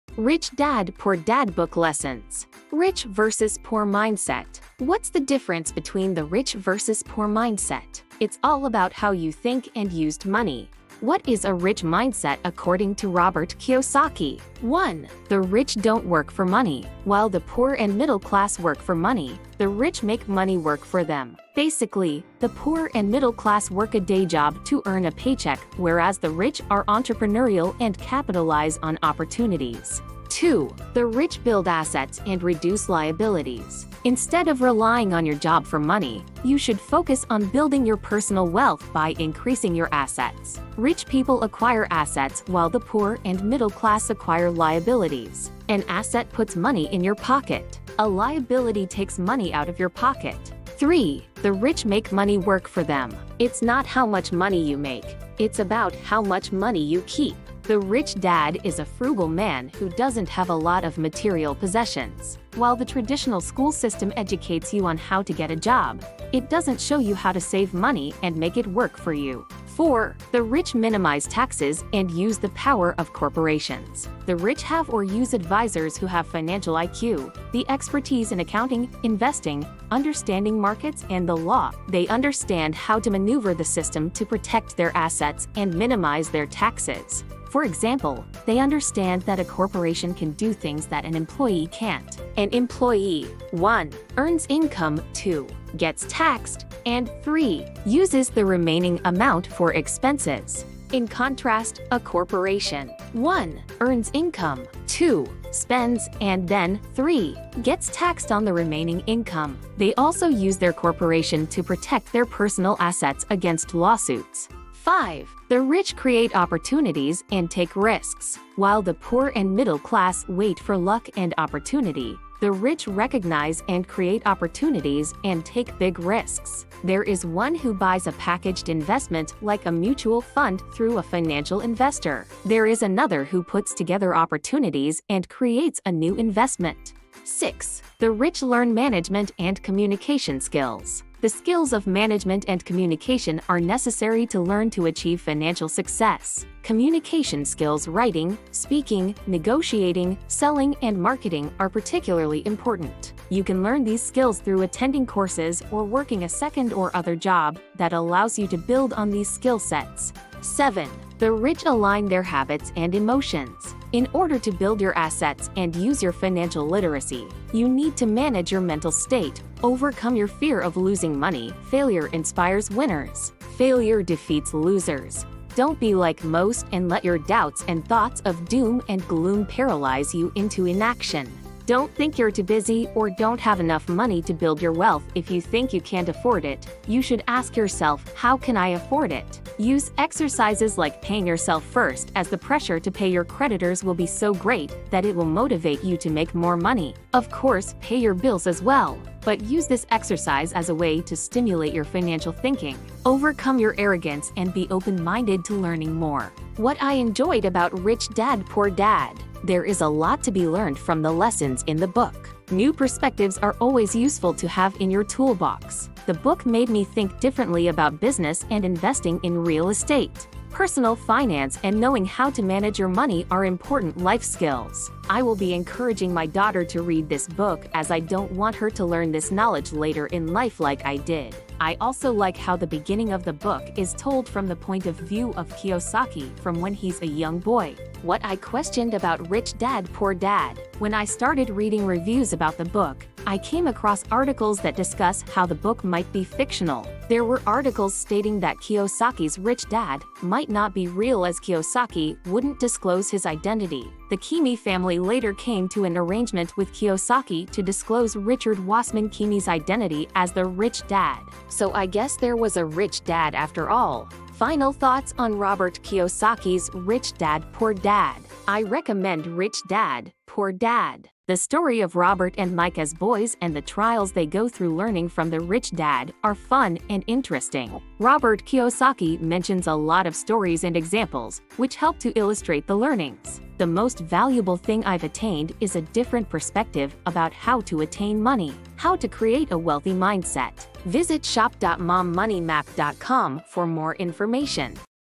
Rich Dad Poor Dad Book Lessons
🌟 Dive into the timeless wisdom of "Rich Dad Poor Dad" and unlock key lessons that can transform your approach to wealth and financial independence. 💰📖

Robert Kiyosaki's book is a game-changer, offering insights that reshape perspectives on money and success.

You can discover the insights and strategies from this book in the full article below. Start implementing these lessons today to pave your path toward financial prosperity and a richer future! 🚀💡

Thanks for stopping by! Your support is appreciated.
- Jacqueline
(spreadsheets, printables and other digital downloads)